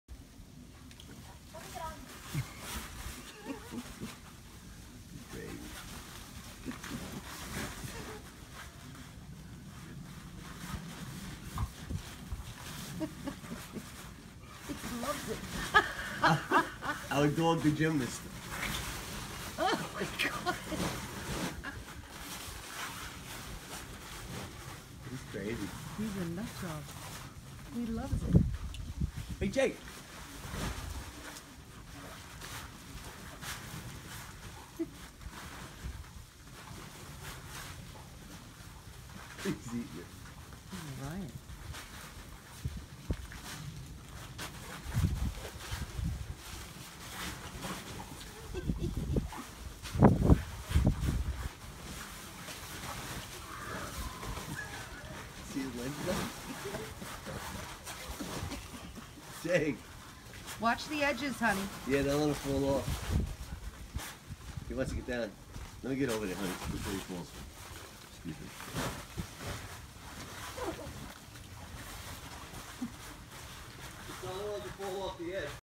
Jake's first snow and on trampoline
Our 9month old black lab Jake seees snow for the first time and plays on the trampoline